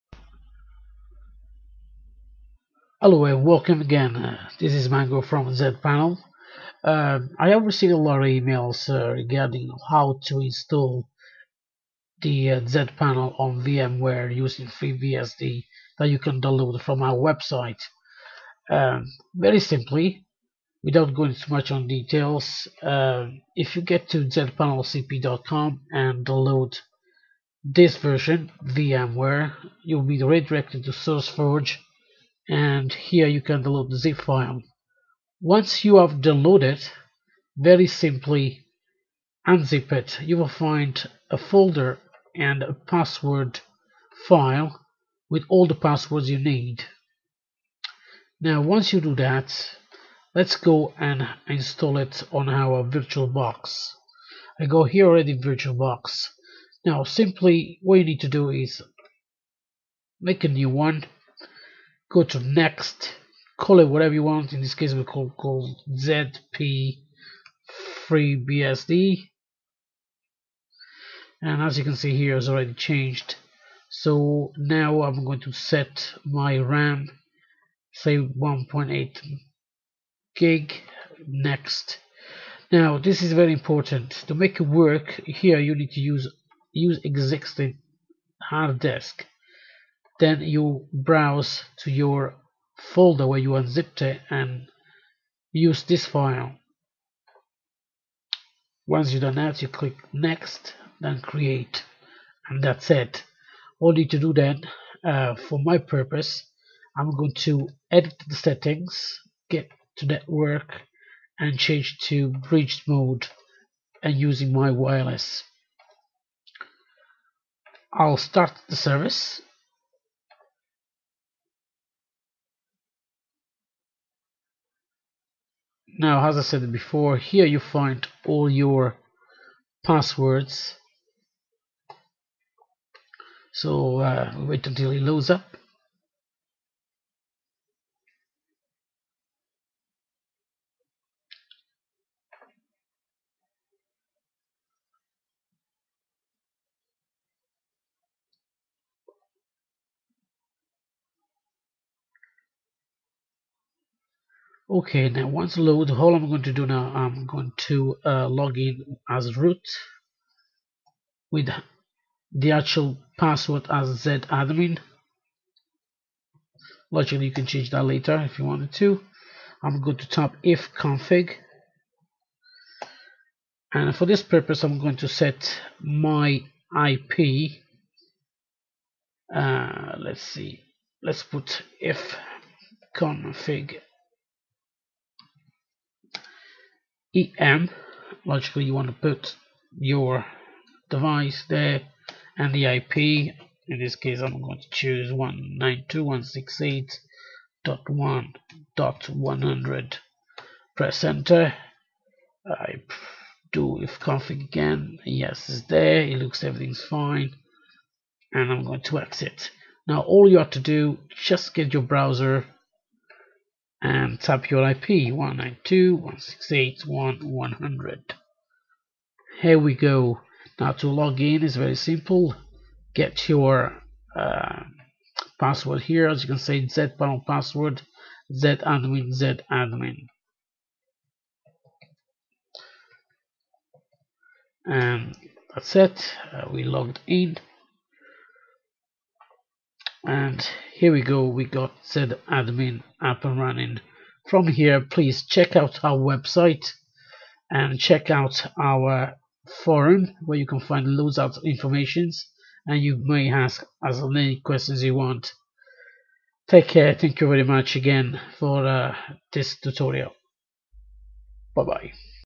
ZPanel installed on VirtualBox
Simple installation of ZPanel on VMware, VirtualBox.
Not much details as it was just to show the basics on how to get started with